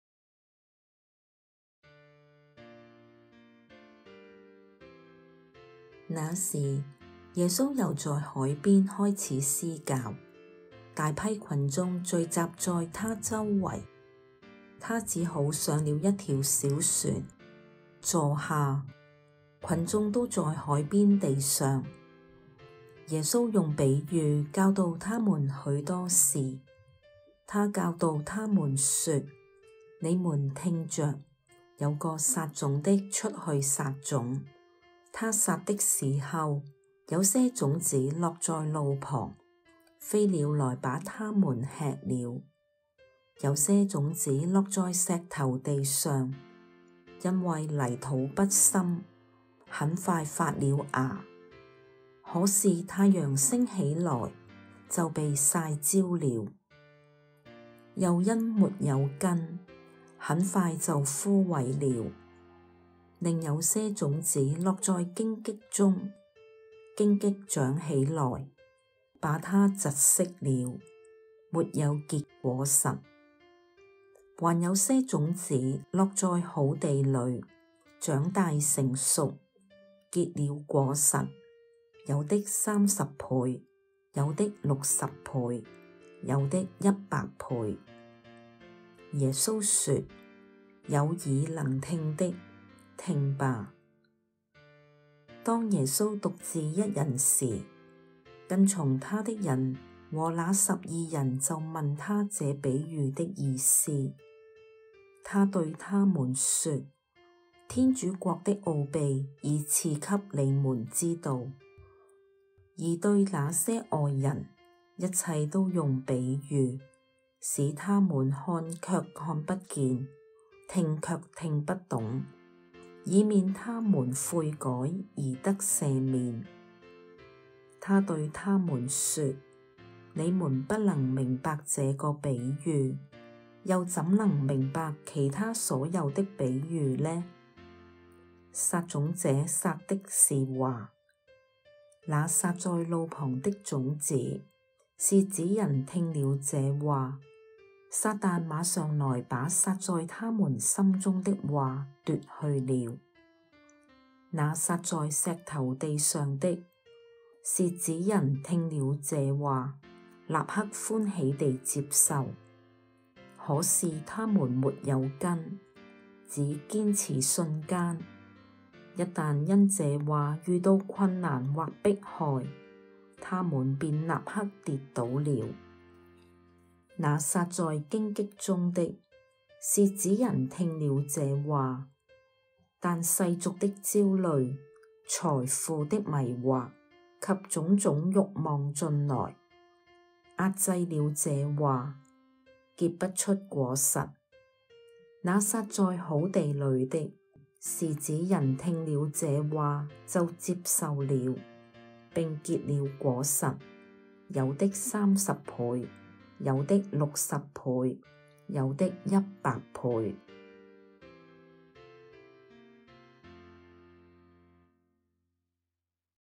常年期第三週 星期三〖馬爾谷福音 4:1-20〗
馬爾谷福音 4:1-20（有個撒種的出去撒種。）

那時，耶穌又在海邊開始施教。大批群眾聚集在祂周圍，祂只好上了一條小船，坐下；群眾都在海邊地上。耶穌用比喻教導他們許多事。祂教導他們說：「你們聽著！有個撒種的出去撒種。他撒的時候，有些種子落在路旁，飛鳥來把它們吃了。有些種子落在石頭地上，因為泥土不深，很快發了芽，可是太陽升起來，就被曬焦了；又因沒有根，很快就枯萎了。另有些種子落在荊棘中，荊棘長起來，把它窒息了，沒有結果實。還有些種子落在好地裡，長大成熟，結了果實，有的三十倍，有的六十倍，有的一百倍。」耶穌說：「有耳能聽的，聽吧！」當耶穌獨自一人時，跟從祂的人和那十二人就問祂這比喻的意思。祂對他們說：「天主國的奧秘已賜給你們知道，而對那些外人，一切都用比喻，使他們看卻看不見，聽卻聽不懂，以免他們悔改而得赦免。」祂對他們說：「你們不能明白這個比喻，又怎能明白其他所有的比喻呢？撒種者撒的是話。那撒在路旁的種子，是指人聽了這話，撒旦馬上來把撒在他們心中的話奪去了。那撒在石頭地上的，是指人聽了這話，立刻歡喜地接受，可是他們沒有根，只堅持瞬間，一旦因這話遇到困難或迫害，他們便立刻跌倒了。那撒在荊棘中的，是指人聽了這話，但世俗的焦慮、財富的迷惑及種種欲望進來，遏制了這話，結不出果實。那撒在好地裡的，是指人聽了這話就接受了，並結了果實，有的三十倍，有的六十倍，有的—百倍！」